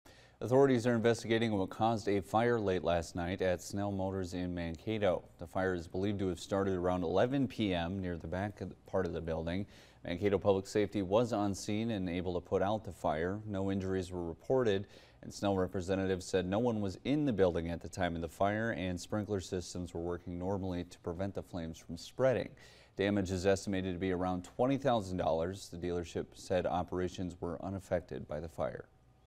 Vehicle fire causes $20,000 in damages at Snell Motors
The fire is believed to have started at around 11 p.m. Wednesday near the back part of the Snell Motors building.